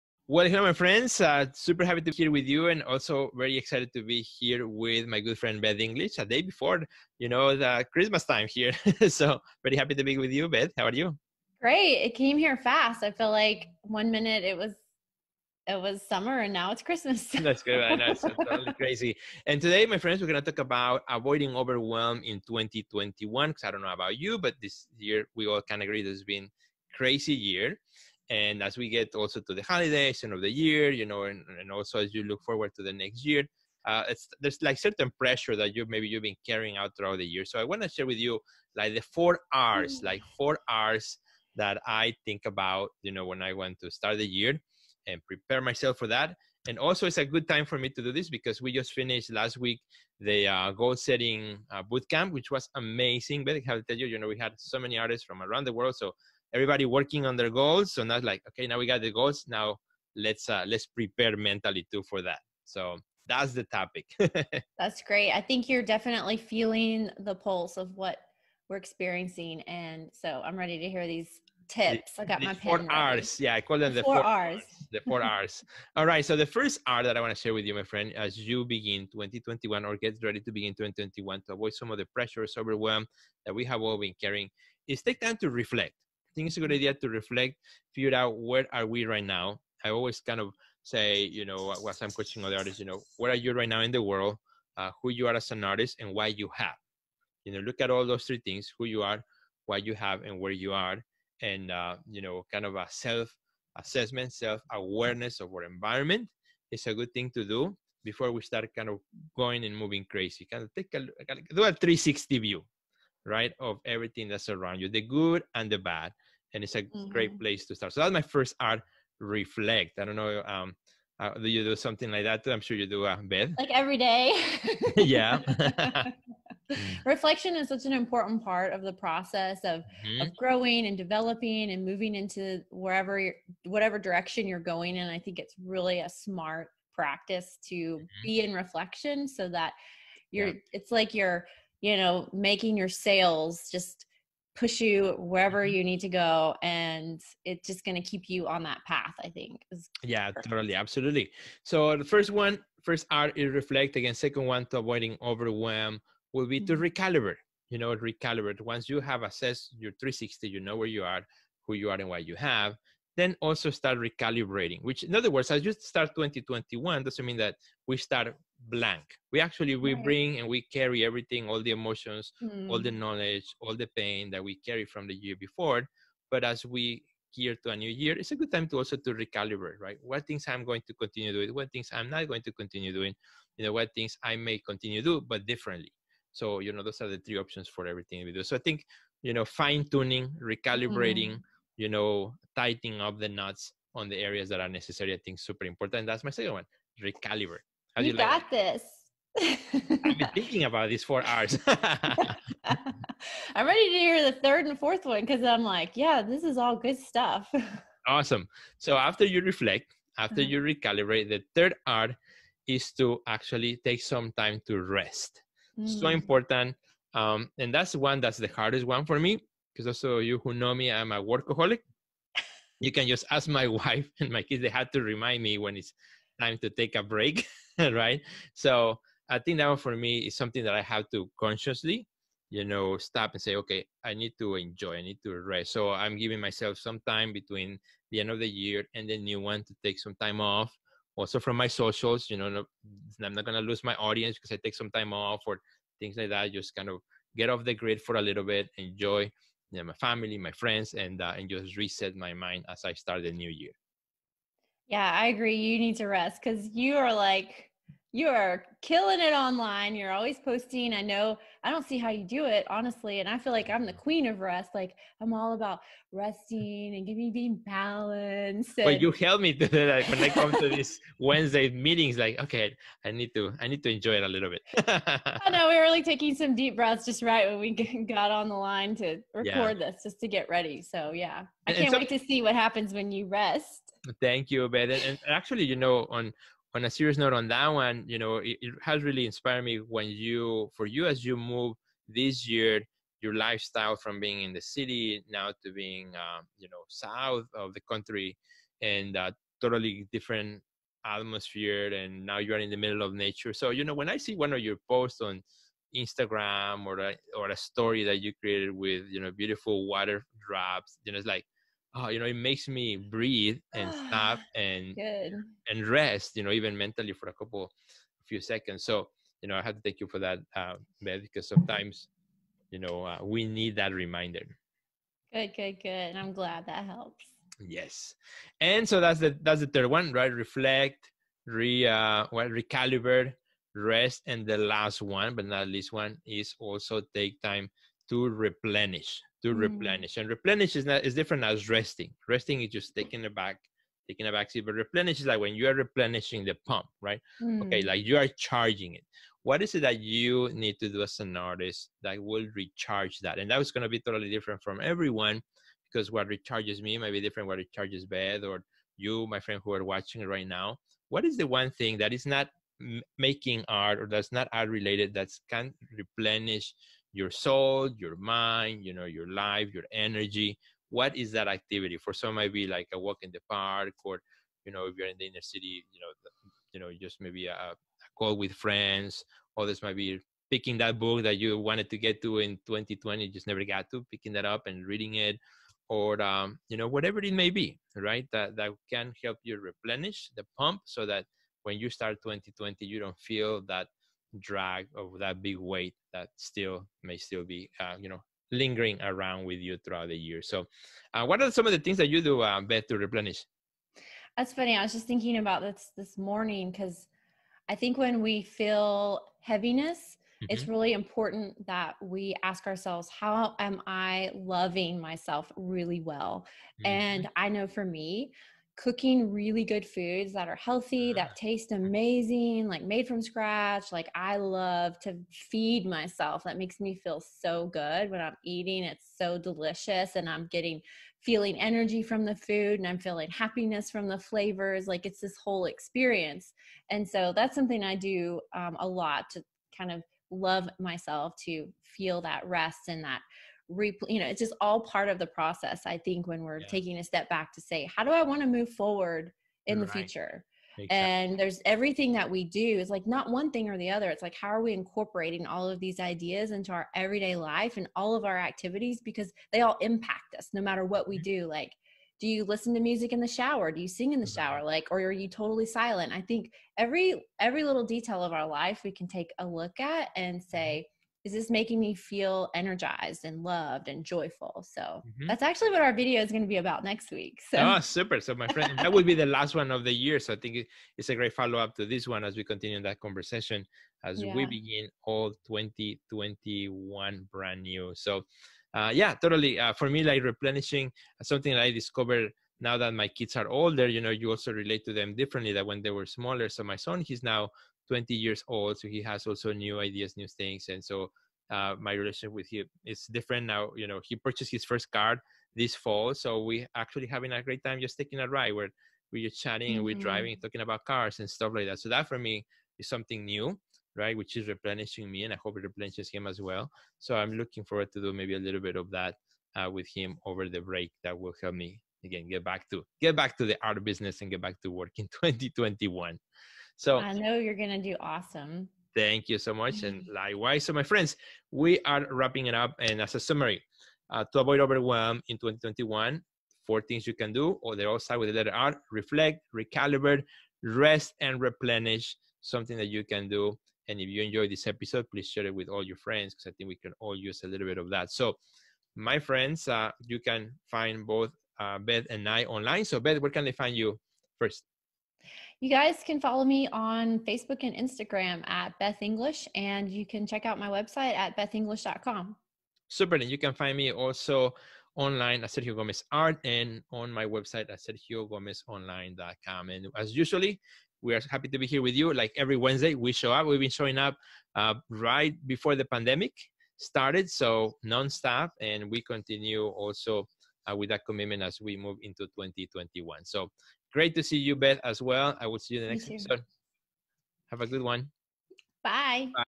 Avoiding Overwhelm in 2021
As we get ready to end this year and start a new one, let's talk about overwhelm, an experience many artists are very familiar with. In this episode with Beth Inglish I share the 4 R's to help you avoid overwhelm in the new year. Let us know what you think na share this episode with your artists friends.

WORK WITH ME AND TAKE YOUR ART CAREER TO THE NEXT LEVEL. CLICK TO GET STARTED: